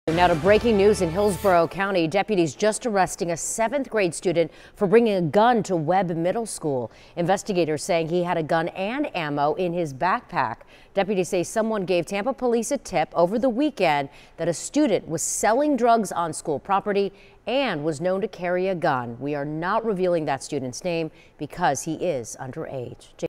Deputies: 7th-grade student brought gun, ammunition to school in Hillsborough Co.
A 7th-grade student in Hillsborough County was arrested Monday after a pistol and ammunition was discovered in his backpack on campus.